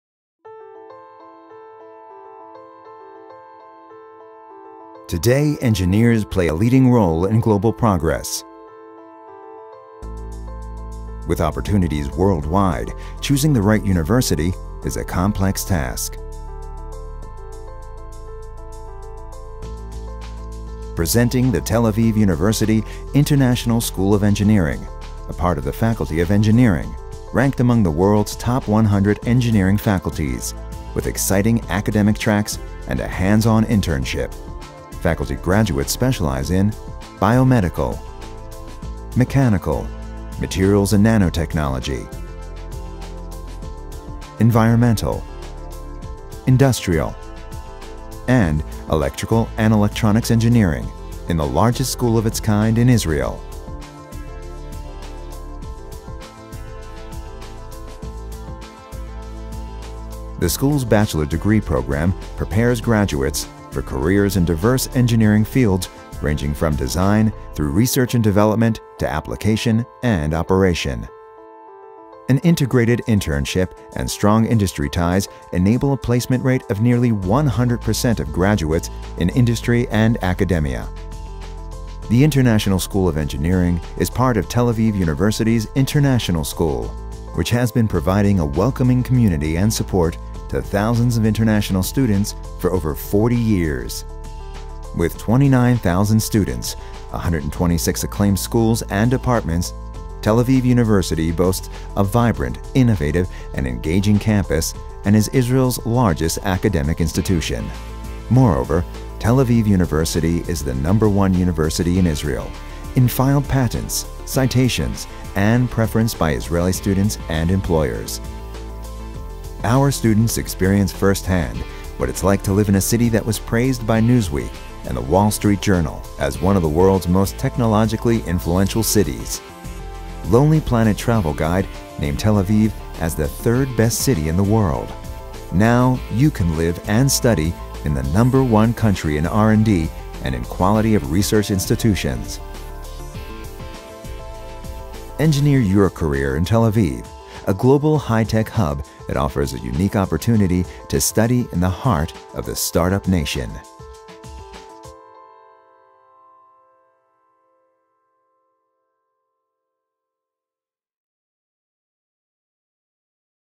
Tel Aviv University Electrical & Electronics Engineering Degree Program
Taught entirely in English, Tel Aviv University's Bachelor of Science Degree program in Electrical & Electronics Engineering builds on Tel Aviv University's rich academic tradition, extensive relation to the high tech industry, and diverse offerings for international students.

Tel Aviv University's School of Electrical Engineering is the birthplace of free internet phone calls and flash drives, the power that drives cancer-detection medical imaging devices and the technology behind NASA's on-board solar energy systems. As Israel's largest and most influential engineering school -- with 30 laboratories, a diverse and distinguished faculty, the largest concentration of master's and doctoral students, and the highest acceptance thresholds in the country -- it is our mission to educate and qualify the future tech leaders of the 21st century.

Highlights of the B.Sc. program include:
• Internationally ranked top-100 Engineering Faculty with world renowned professors
• A integrated internship program at Israel's leading high tech and start-up firms
• Intimate class size & competitive tuition rates
• Program taught entirely in English

Beyond the classroom, TAU's lively, verdant campus is a home away from home for the nearly 30,000 students, including over 1,000 international students, enrolled in the university's 105 departments and 27 schools spanning 9 faculties. The campus includes several libraries, a modern sports center open to all students, an active Student Union that offers a wide range of student services, three museums, and a vast array of cultural activities.  Furthermore, downtown Tel Aviv — an international hotspot of creativity, culture, innovation, and fun — is only a brief bike ride away from campus.

Applications for the 2013-14 academic year are currently being accepted.